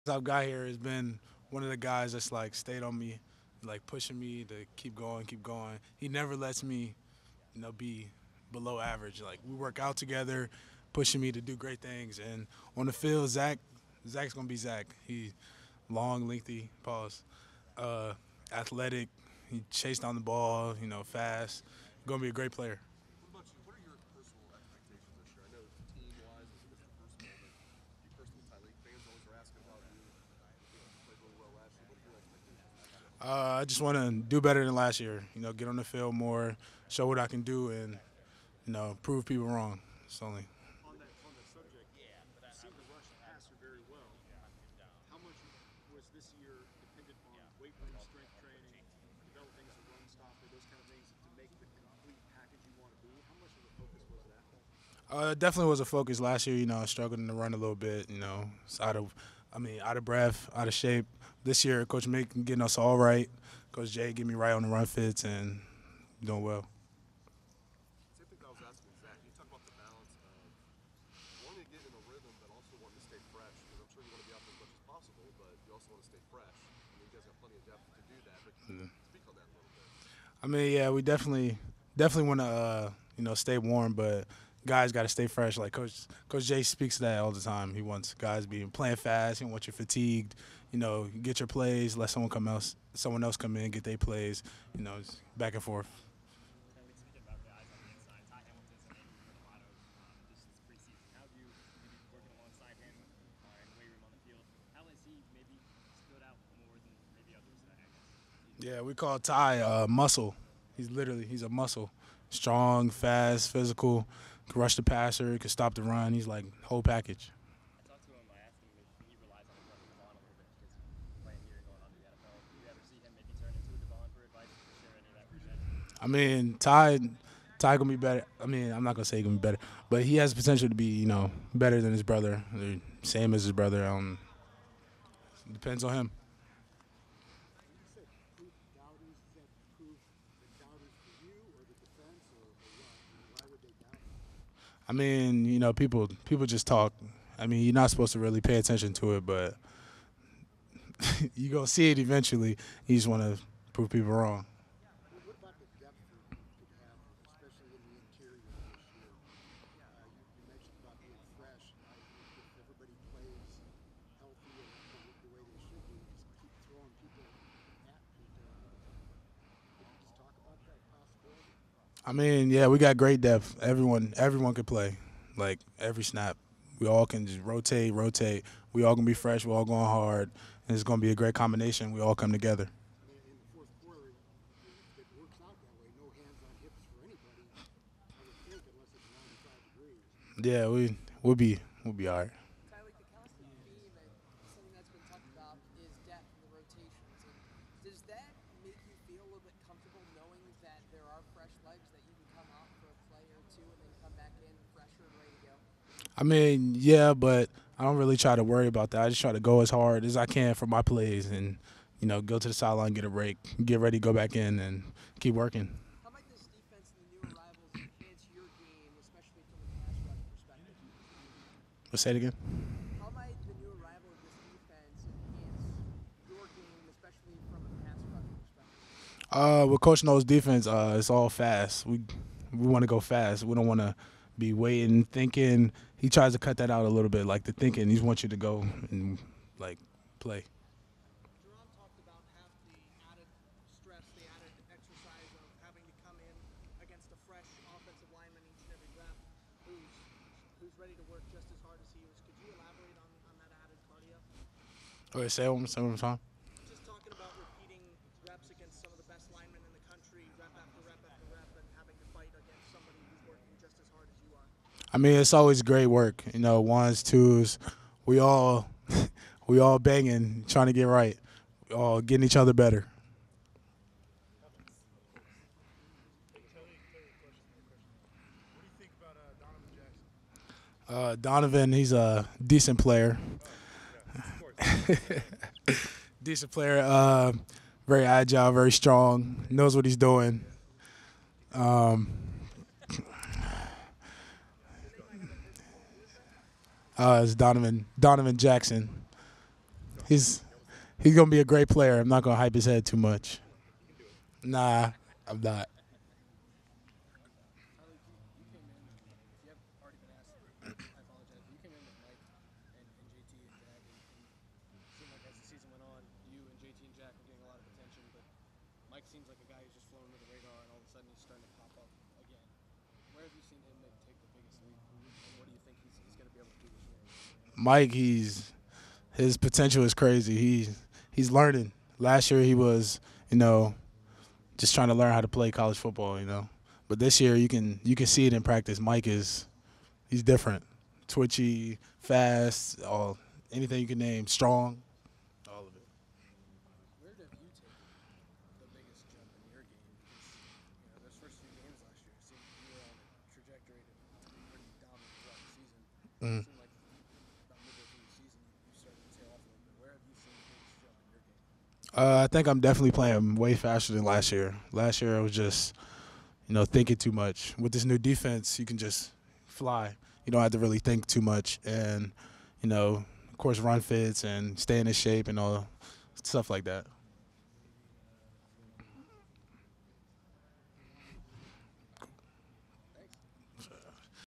Tyleik Williams: Ohio State defensive tackle breaks down progress in camp
Ohio State defensive tackle Tyleik Williams breaks down his progress along the defensive line for the Buckeyes.

Ohio State Buckeyes videos from Columbus, Ohio from the staff of Lettermen Row.